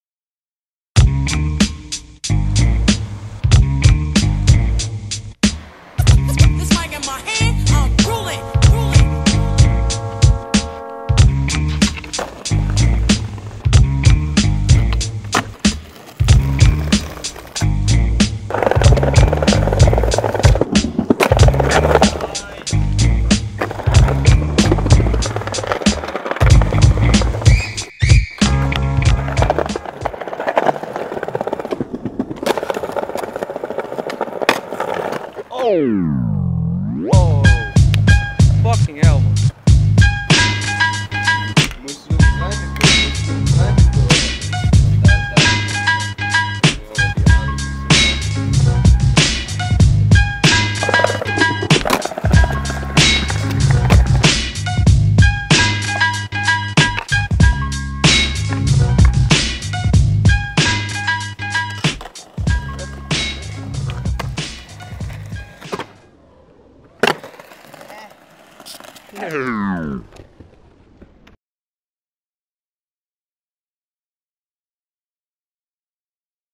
Clippie met Flippie - EP 1 - Vledder Hallen
Goed gaan met Bassie uit Meppel. In de eerste episode skate Flippie de Vledder Hallen van Meppel. Enjoy!